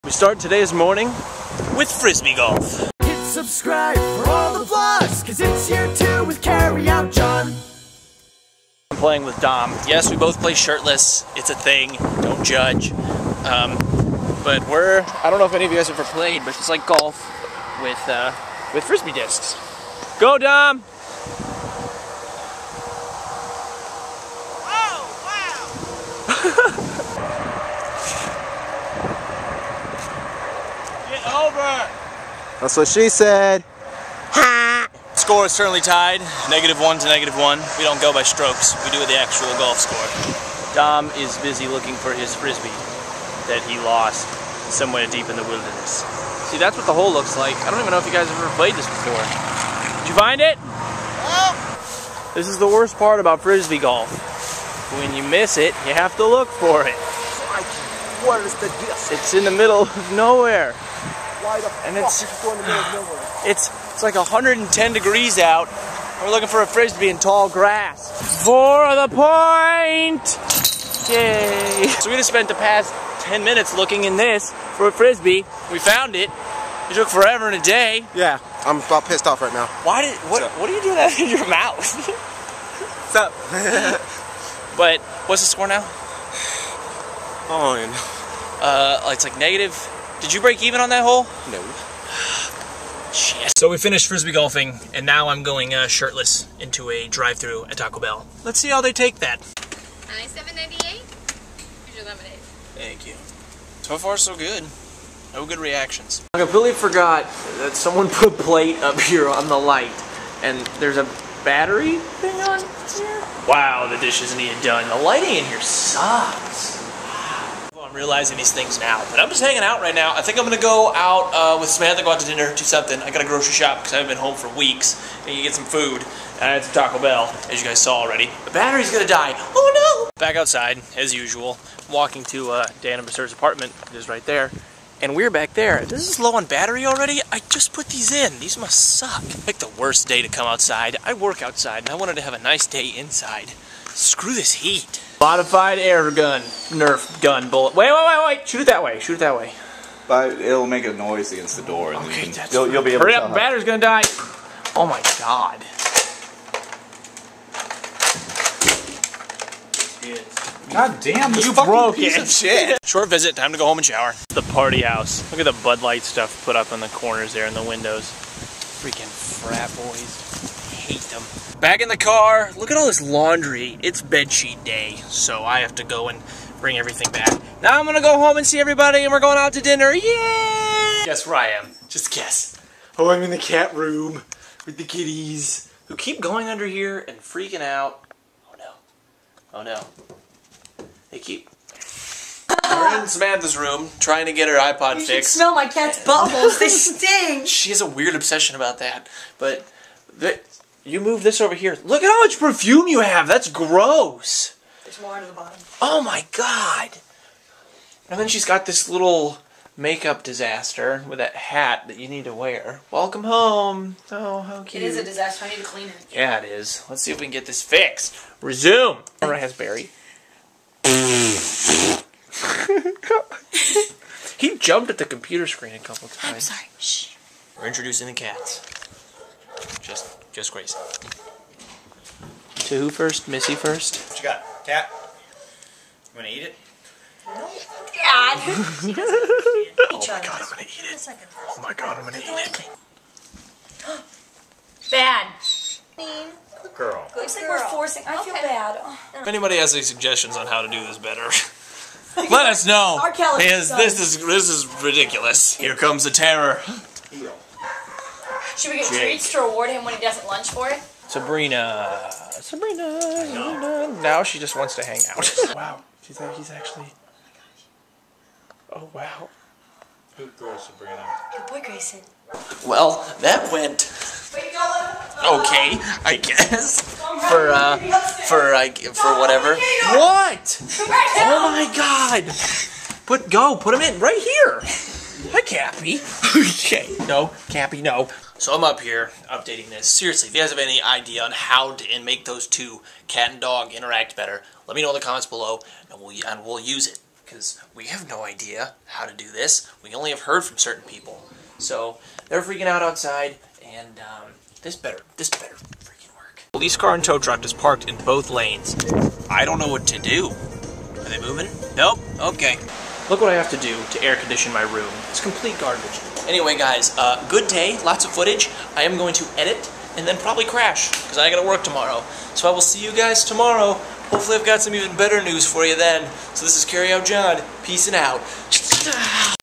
THE DRUNK HOUSE (8.2.11 - Day 8)
We tried introducing the cats to the dogs today, and played frisbee golf!